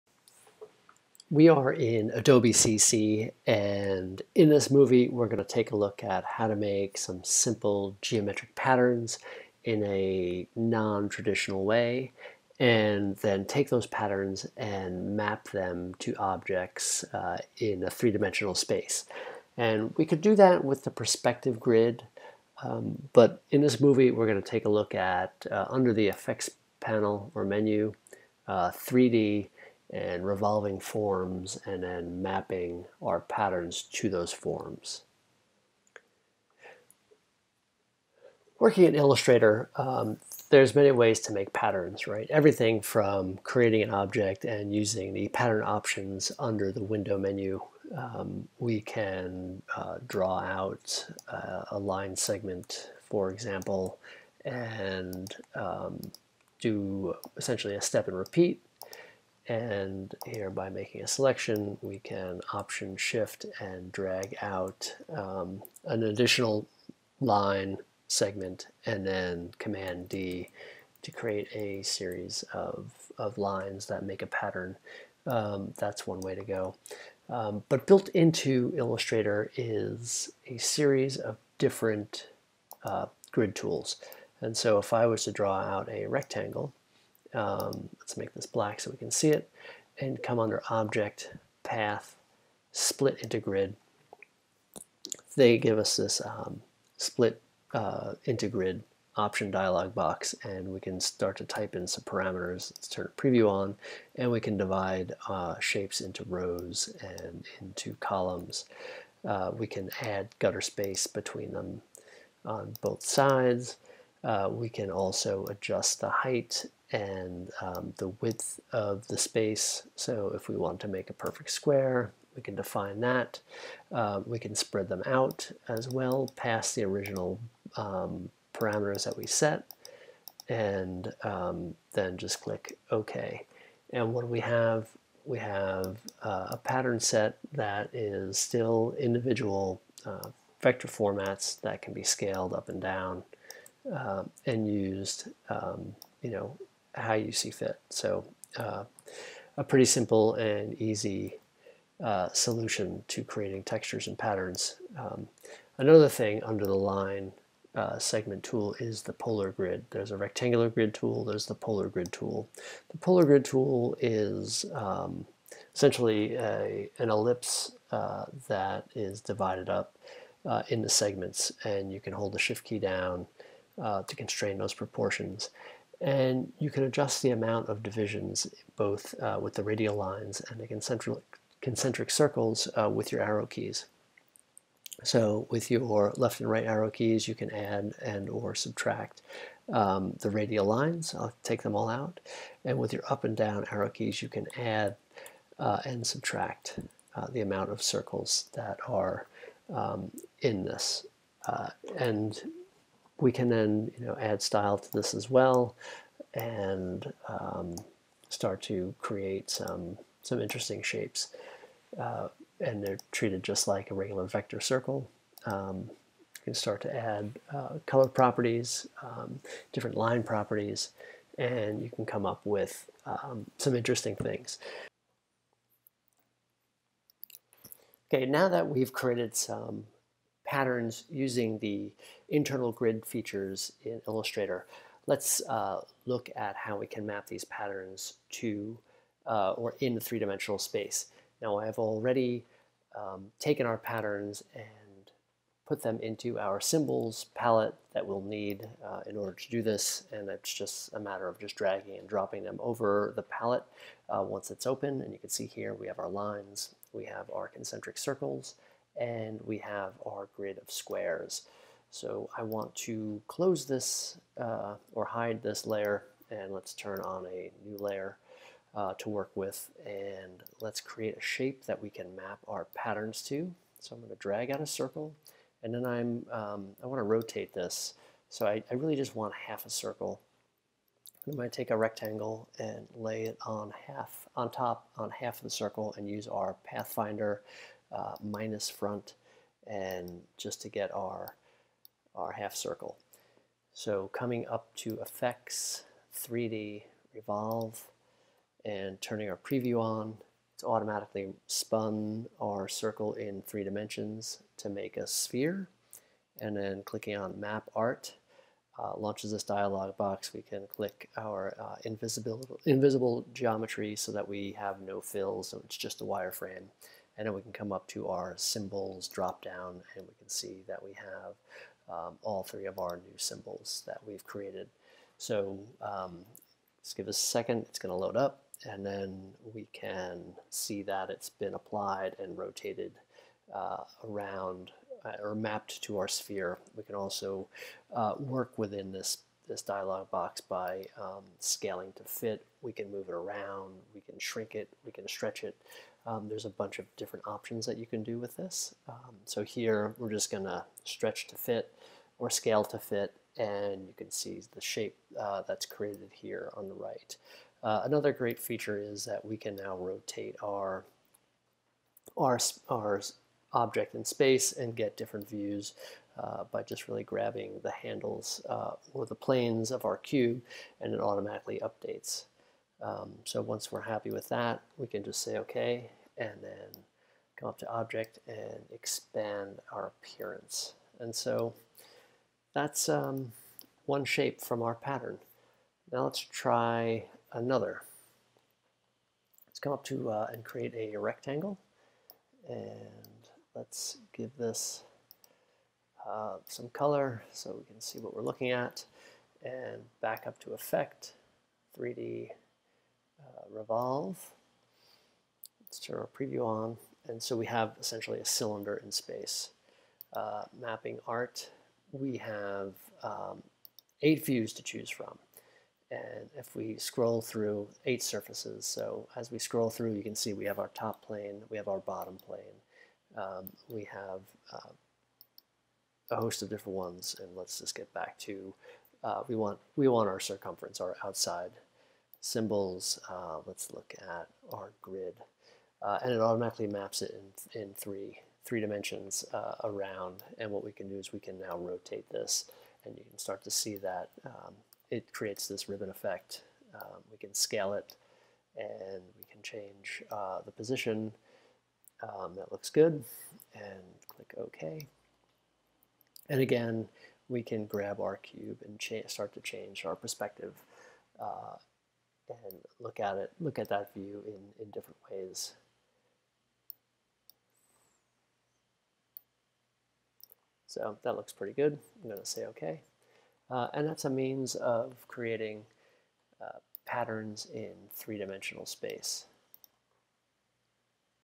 Illustrator cc Grid tools and Mapping art in 3-D
Creating pattern art withAdobe Illustrator cc grid tools and mapping art to create 3D effects.